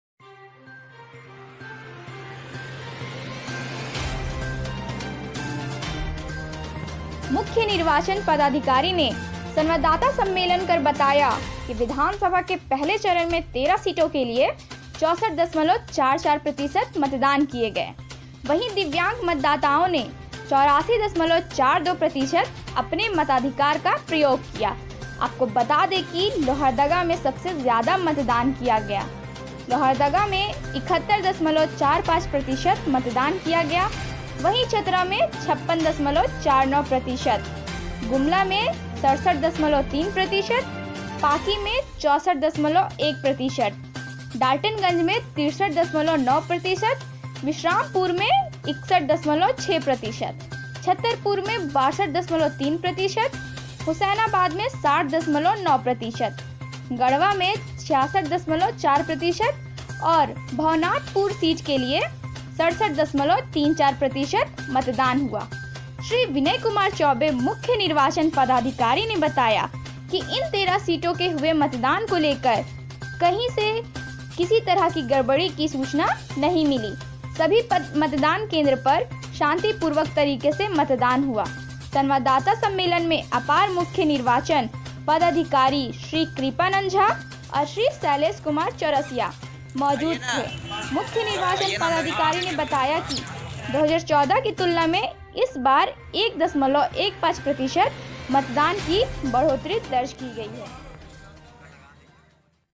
मुख्य निर्वाचन पदाधिकारी ने संवाददाता सम्मेलन कर बताया के विधानसभा के पहले चरण में 13 सीटों के लिए
मुख्य निर्वाचन पदाधिकारी ने संवाददाता सम्मेलन कर बताया  के विधानसभा के पहले चरण में 13 सीटों के लिए 64.44% मतदान किए गए वही दिव्यांग मतदाताओं ने 84.42 प्रतिशत अपने मताधिकार का प्रयोग किया आपको बता दें कि लोहरदगा में सबसे ज्यादा मतदान किया लोहरदगा में 71.45 प्रतिशत मतदान किया गया वही चतरा में 56.49% गुमला में 67.3 प्रतिशत पाकी में 64.1 डालटेनगंज में 63.9 विश्रामपुर में 61.6 छतरपुर में 62.3 हुसैनाबाद में 60.9 गढ़वा में 66.4 और भवनाथपुर सीट के लिए 67.34% मतदान हुआ श्री विनय कुमार चौबे मुख्य निर्वाचन पदाधिकारी ने बताया कि इन 13 सीटों के हुए मतदान को लेकर कहीं से किसी तरह की गड़बड़ी की सूचना नहीं मिली सभी मतदान केंद्र पर शांतिपूर्वक तरीके से मतदान हुआ संवाददाता सम्मेलन में अपार मुख्य निर्वाचन पदाधिकारी श्री कृपानंद झा और श्री शैलेश कुमार चौरसिया मौजूद थे मुख्य निर्वाचन पदाधिकारी ने बताया कि 2014 की तुलना में इस बार 1.15 प्रतिशत  मतदान की बढ़ोतरी दर्ज की गई है